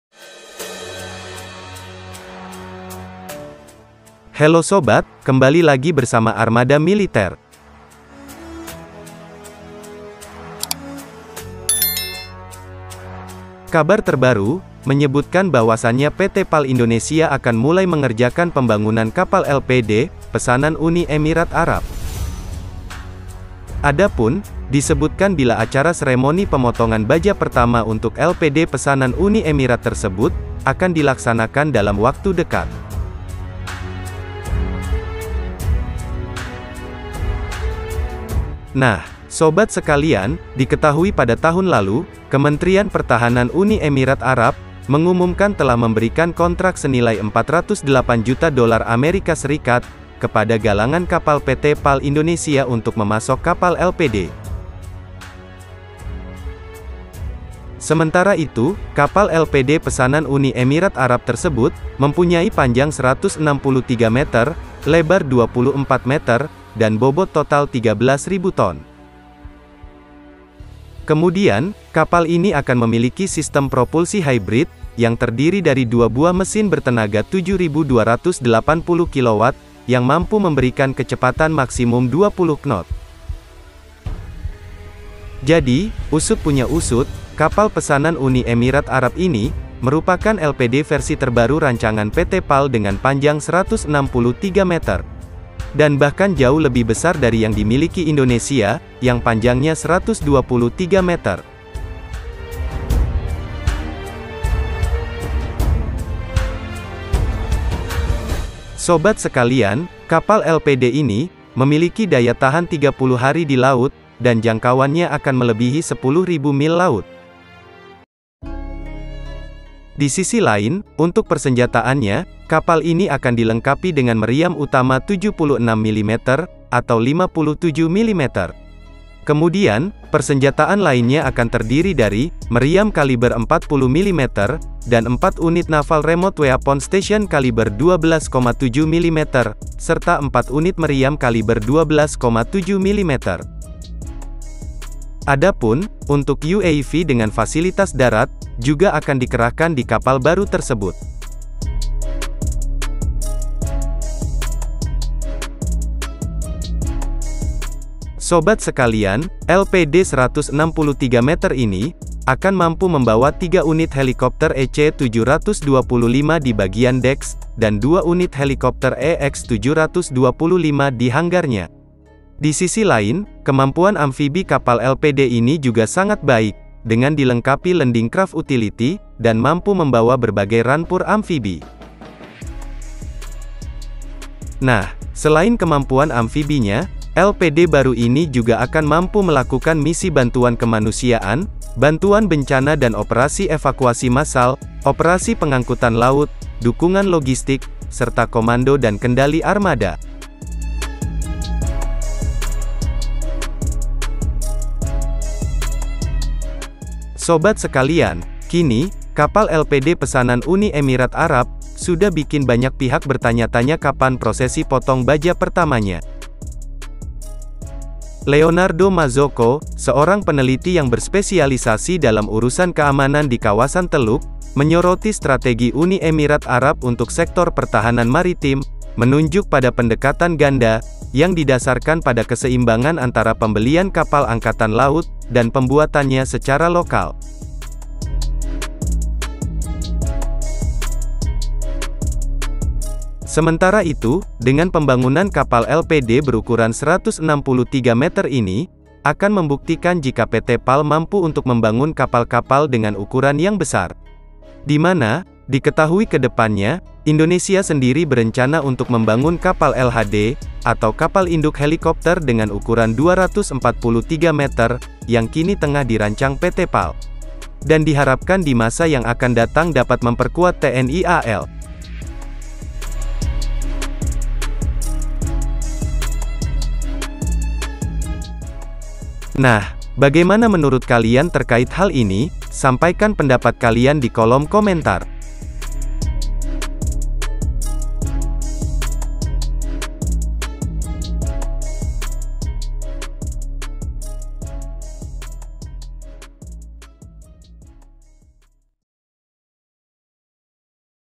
PT PAL Indonesia Akan Mulai Bangun Kapal LPD 163 Meter Pesanan UEA - Kontrak Senilai 408 juta USD
PAL Indonesia Akan Mulai mengerjakan pembangunan kapal LPD pesanan UEA.

Diketahui pada tahun lalu, Kementerian Pertahanan Uni Emirat Arab (UEA) mengumumkan telah memberikan kontrak senilai AED 1,5 miliar (US$ 408,32 juta) kepada galangan kapal PT PAL Indonesia untuk memasok multimission vessel.

Kapal Multimission Vessel yang dimaksud dibangun dari jenis LPD sepanjang 163 meter.

Desain LPD dari PT PAL yang dipilih UEA merupakan varian 163 meter, merupakan versi yang lebih panjang dari pesanan Angkatan Laut Filipina (123 meter).